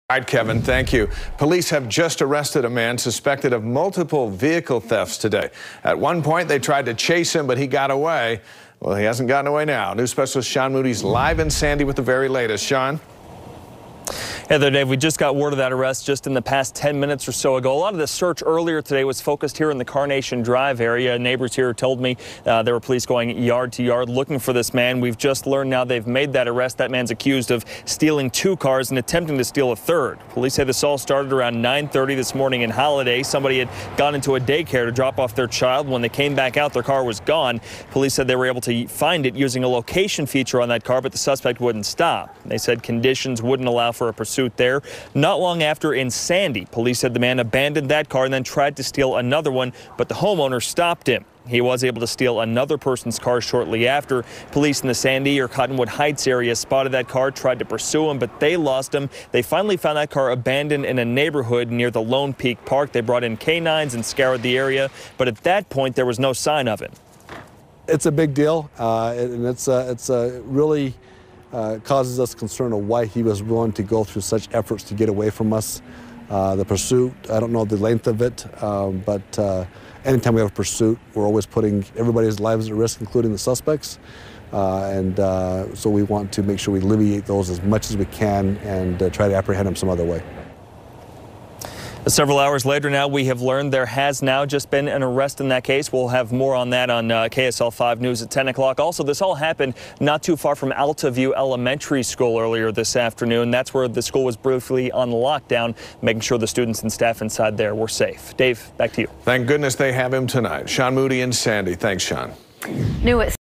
Police arrest man suspected of multiple vehicle thefts
Police arrested a man suspected of multiple vehicle thefts. News Specialist Sean Moody reports.